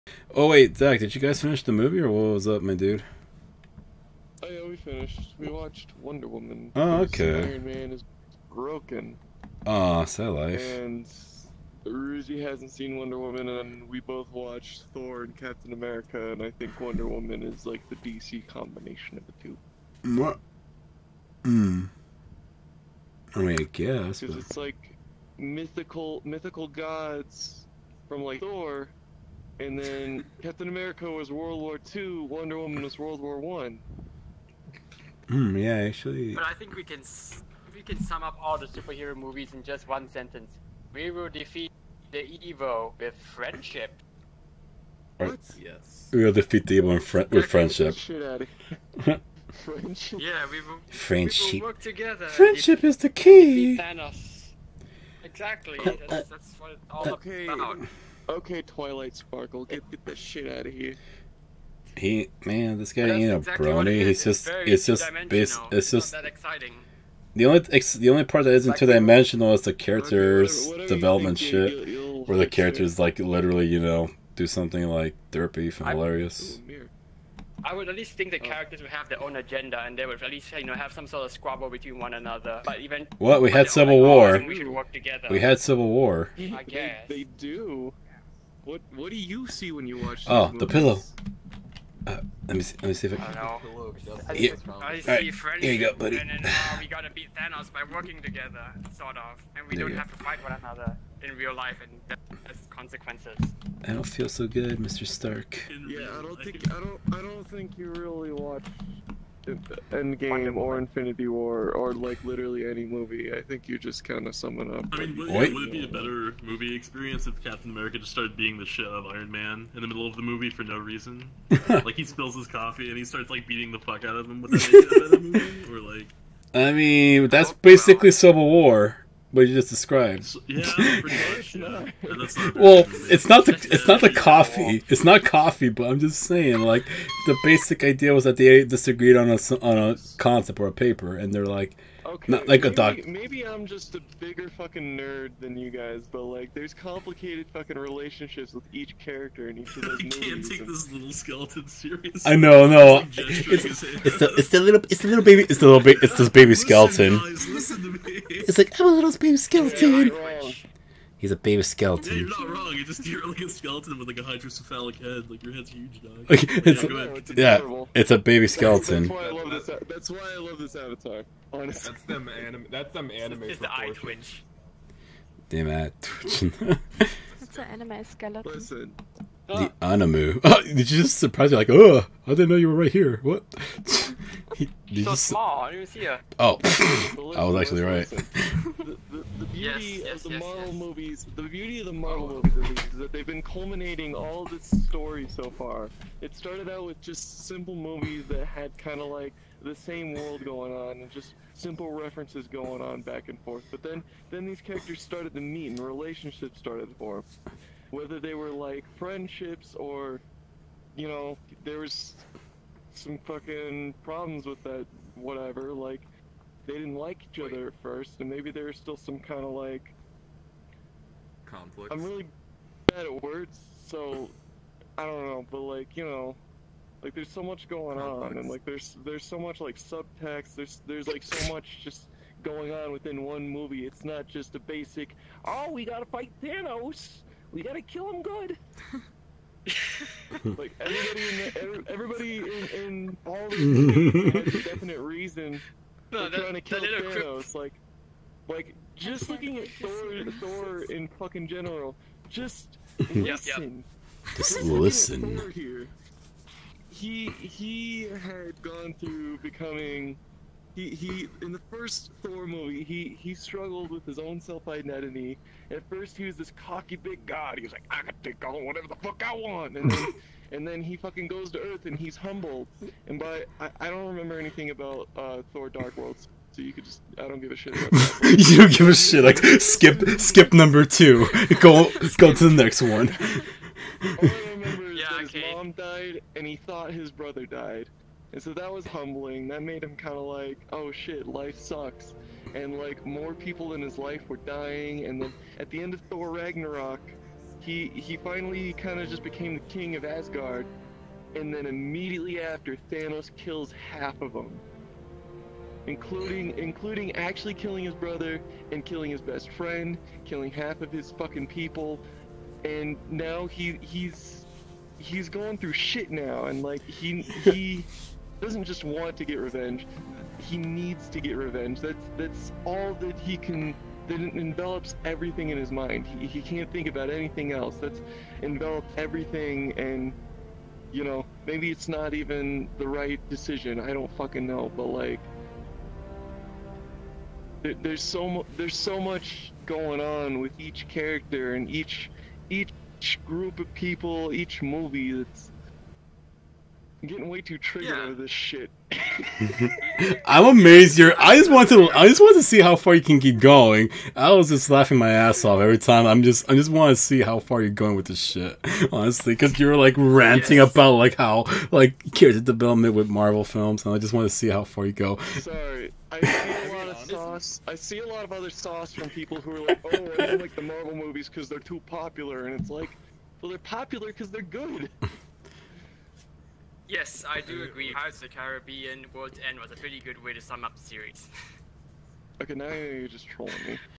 tiny skeleton in VRChat rants about Marvel movies
Just a funny thing on Blaze's stream the other night